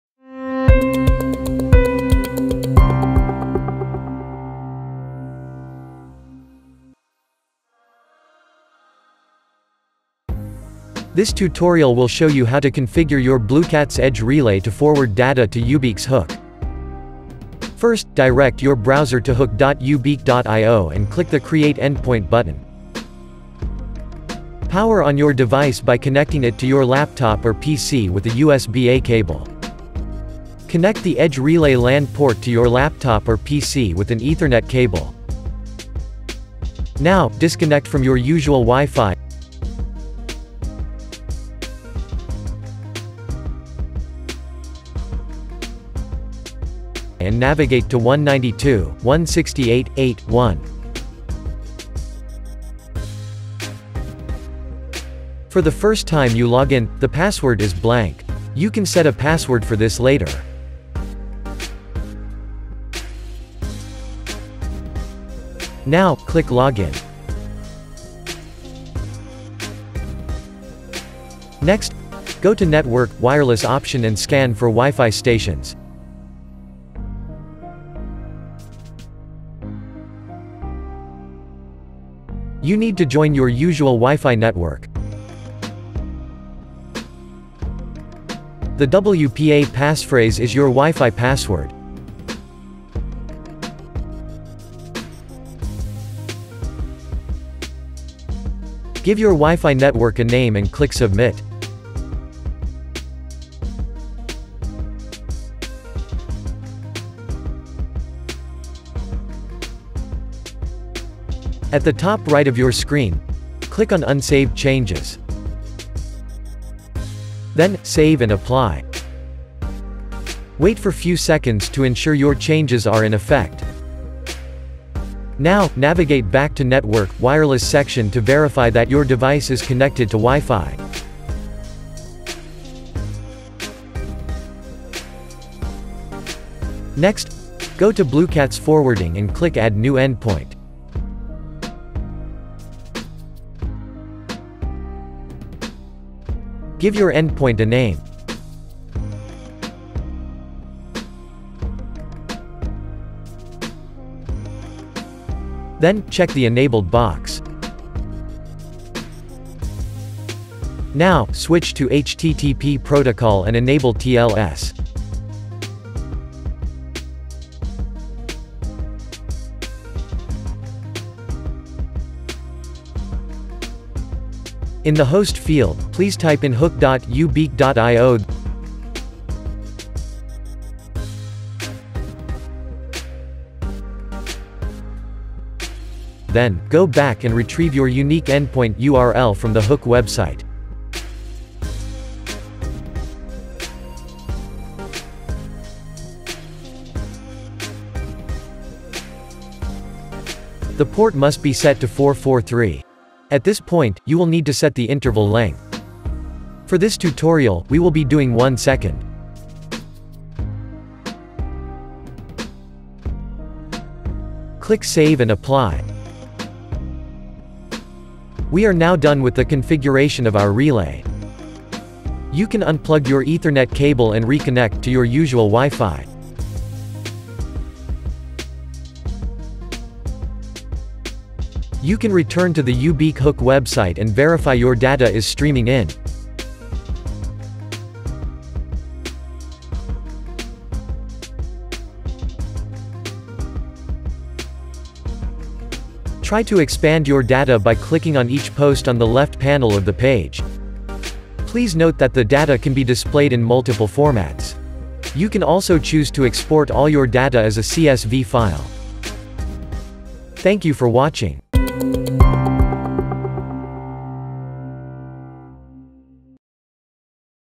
Configuring BlueCat's Edge Relay Bluetooth Gateway to Send HTTP Requests to uBeac
This is a step by step guide on how to configure your Bluecat's Edge Relay Bluetooth Low Energy (BLE) WiFi Gateway to send payload via HTTP(S) requests to uBeac Hook. Hook.uBeac.io is an online  webhook service to debug HTTP(S) requests.